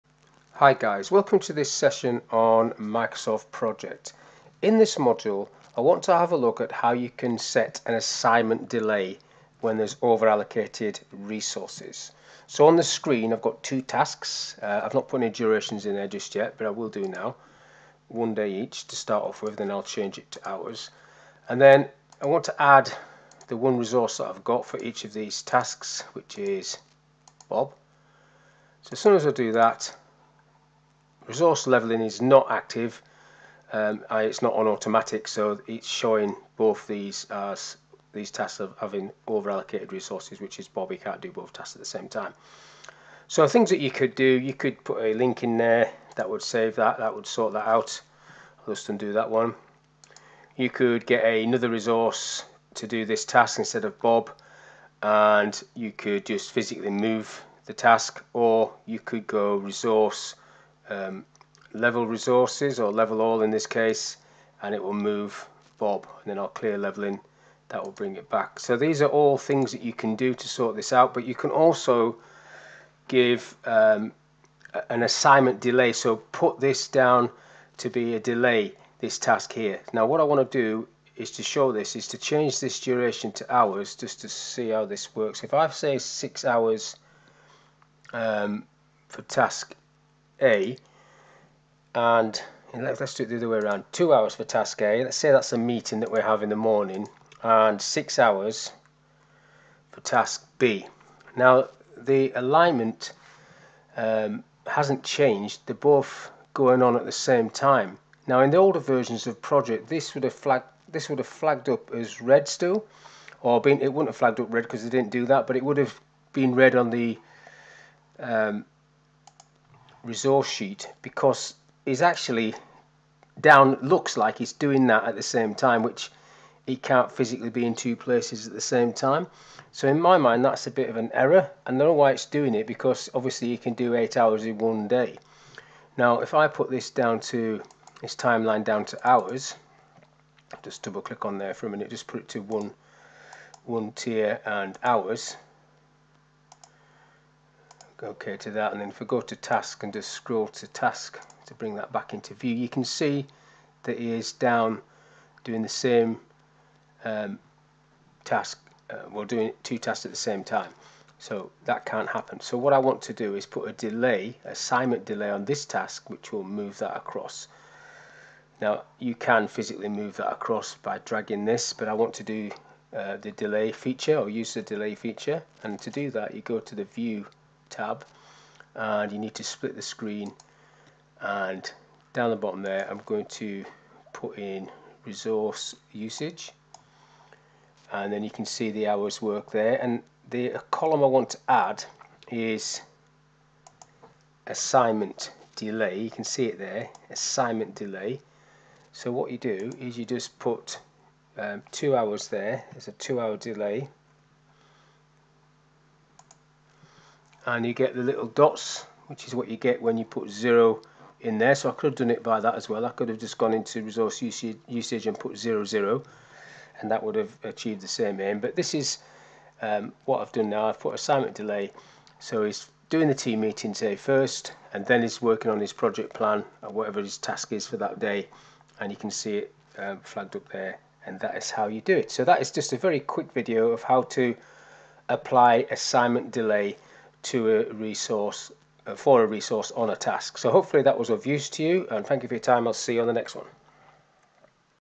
Boost Productivity: Implementing Assignment Delay in Microsoft Project
Boost your project efficiency with this simple trick in Microsoft Project that will save you time and stress!
In this tutorial, you will learn how to increase productivity by implementing assignment delay in Microsoft Project. Follow along step by step to optimize your project scheduling and improve efficiency. Boost your productivity with this useful feature today!

This video explains how to apply an assignment delay in Microsoft Project. Assignment Delay. The tutor demonstrates how to resolve resource over allocation, by levelling resources, moving a task, changing the resource, and linking a task. The tutorial covers how to add an assignment delay. The tutor splits the screen from the view tab and selects the Resource usage view as the bottom half of the screen. The tutor then adds the assignment delay column and sets the delay period. The tutor also changes the Timescale to display hours. This is a Microsoft Project tutorial covering, assignment delay.